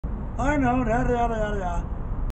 Urination Man "Mike Adamson."
I was reminding Urination Man "Mike Adamson."  why he wasn't allowed back on property.  In this video he was repeating back to me what he understood.